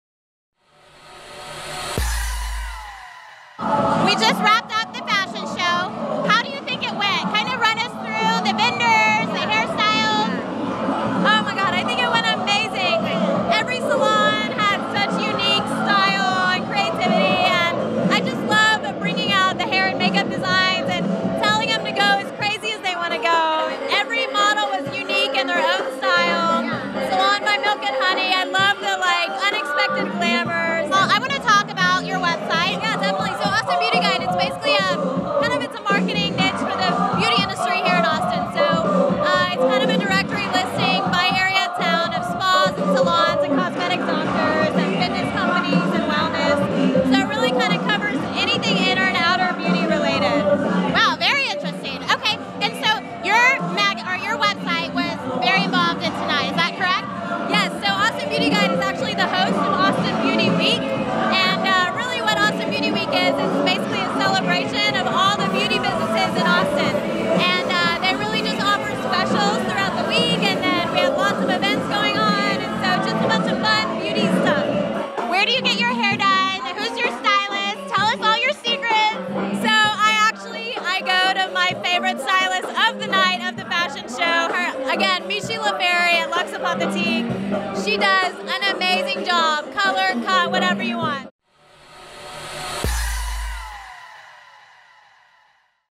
Meredith Davis Reveals Hair Secret
The loop LIVE obtained the secret to Meredith Davis' bomb blonde hair in an exclusive interview with the CEO and founder of Austin Beauty Guide and Austin Beauty Week.

This week Meredith successfully kicked off Austin Beauty Week with an amazing fashion on the rooftop of Terrace 59. Beauty Week is a series of events from April 1-7 showcasing local Austin businesses in the beauty and fashion industry. Click to watch the video